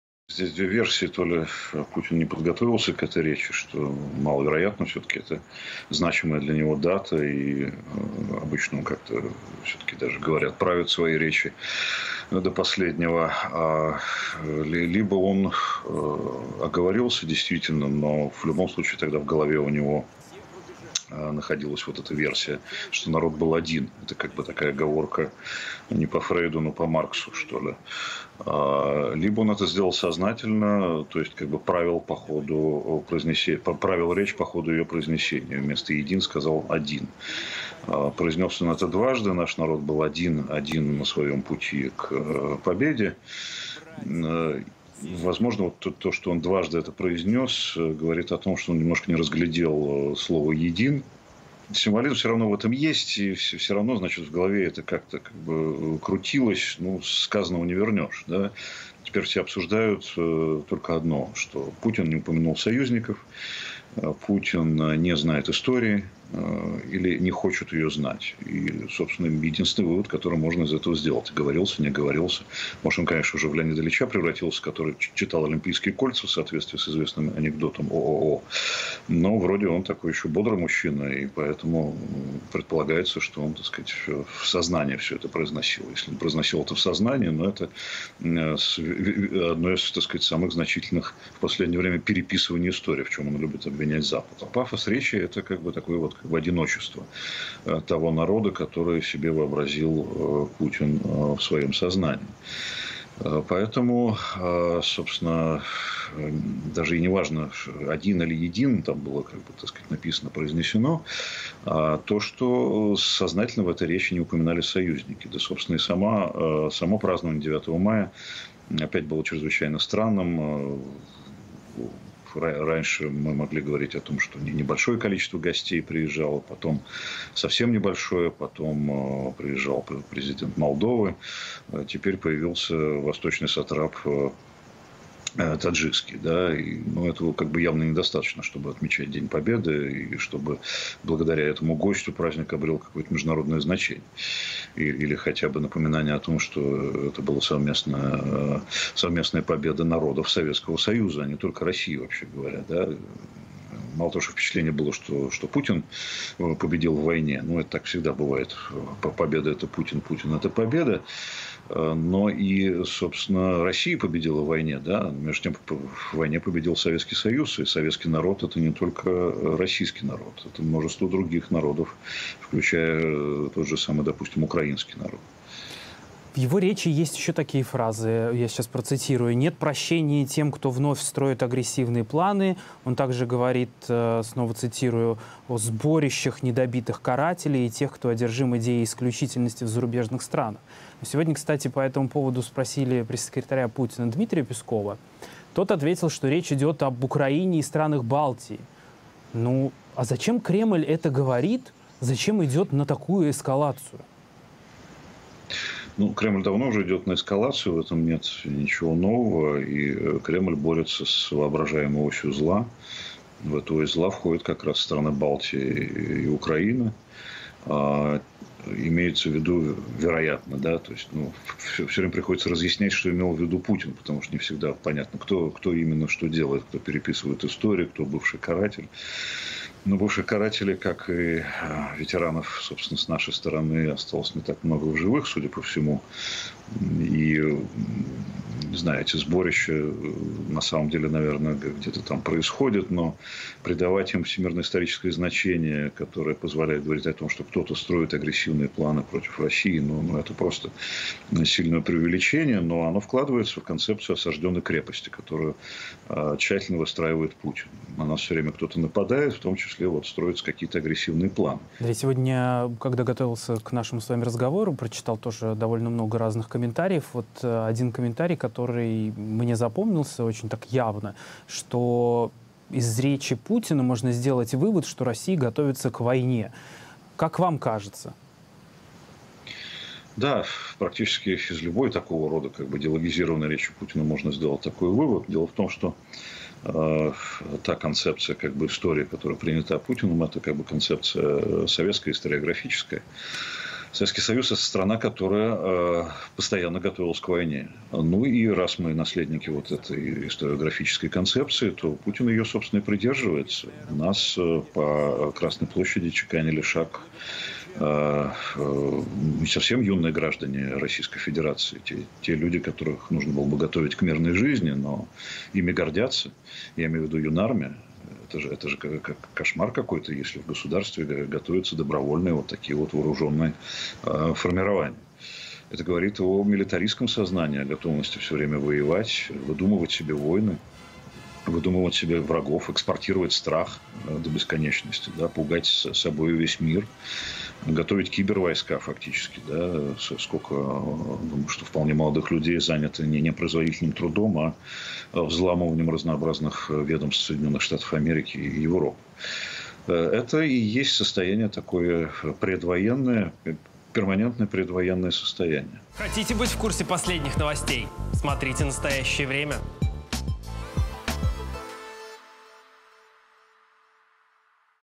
"Перманентное предвоенное состояние". Значение слов Путина о "карателях" и "одном" народе
Владимир Путин изменил одну букву в речи, приготовленной для выступления на Параде Победы в Москве 9 мая, в итоге смысл фразы кардинально изменился. Вместо слова "един" стало "один", поэтому с трибуны прозвучало, что советский народ был один на пути к победе.

"В самое трудное время войны, в решающих сражениях, определивших исход борьбы с фашизмом, наш народ был один, один на многотрудном, героическом и жертвенном пути к победе", – цитирует Путина сайт Кремля. Однако на сайте мэрии Москвы та же цитата приведена со словом "един" – судя по всему, как она была задумана изначально.

Кроме того, в своей речи президент Владимир Путин упомянул "недобитых карателей". "Ни для кого не секрет, что в европейских странах все больше поднимают голову неонацисты, и европейцы с ними борются", – объяснил это выражение пресс-секретарь президента Дмитрий Песков.